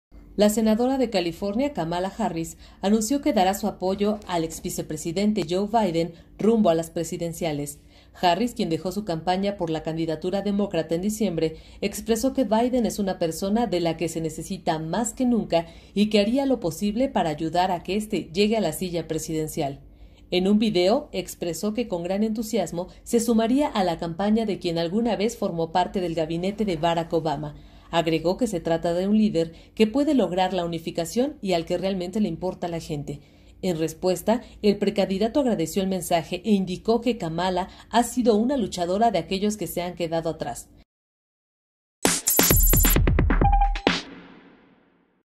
Biden suma apoyos, Kamala Harris se une a su campaña
La senadora de California Kamala Harris anunció que dará su apoyo para el exvicepresidente Joe Biden rumbo a las presidenciales.